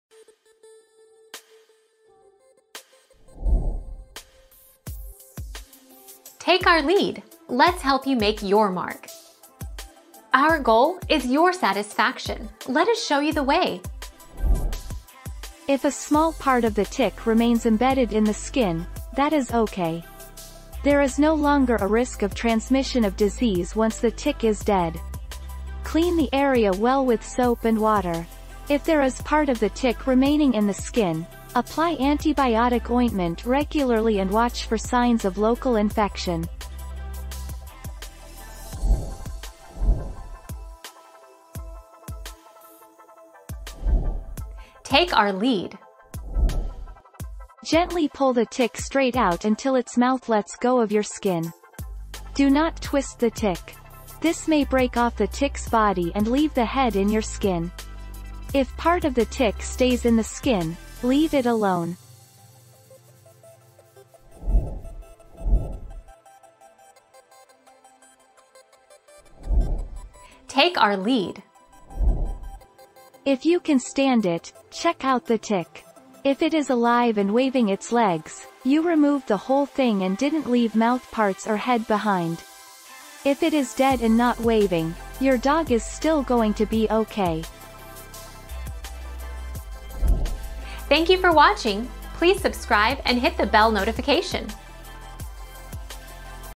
What happens if part of tick is left in skin?
00:00 - What happens if part of tick is left in skin?
00:40 - Is it bad to leave a tick head in your skin?
01:06 - How do you know if the whole tick is out?

Laura S. Harris (2020, December 31.) What happens if part of tick is left in skin?
    AskAbout.video/articles/What-happens-if-part-of-tick-is-left-in-skin-202545